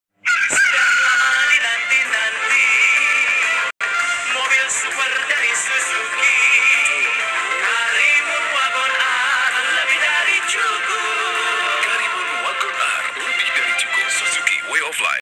Iklan Suzuki Karimun Wagon R - Super Efficient (2013) @ Trans TV, RCTI, Kompas TV, Indosiar, & SCTV
Iklan ini pernah ditayangkan di Trans TV, RCTI, Kompas TV, Indosiar, & SCTV pada tahun 2013.
Featuring: Denny Cagur.